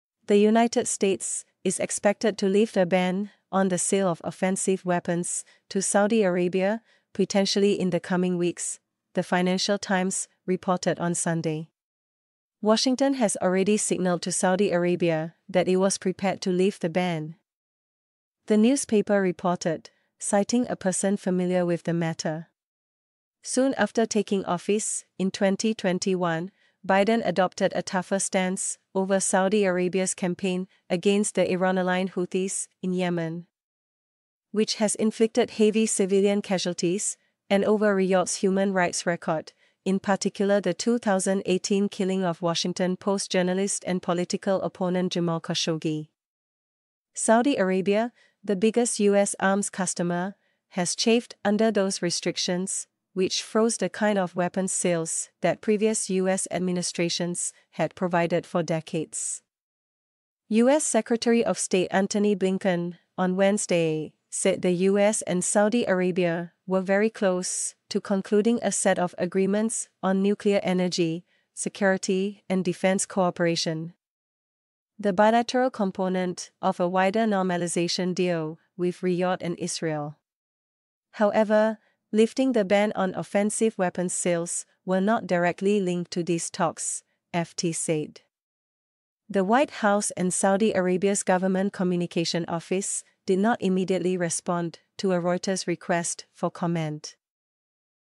US expected to lift ban on sale of offensive weapons to Saudi Arabia, FT reports
💗📌 Singapore